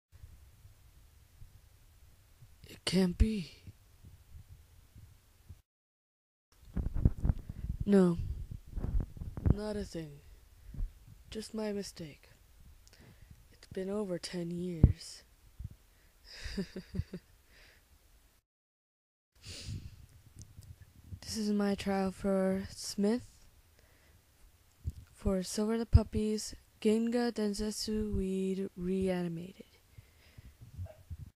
smith try out
i am try out for smith

if you listen to the last part of the vid you'll hear a dog bark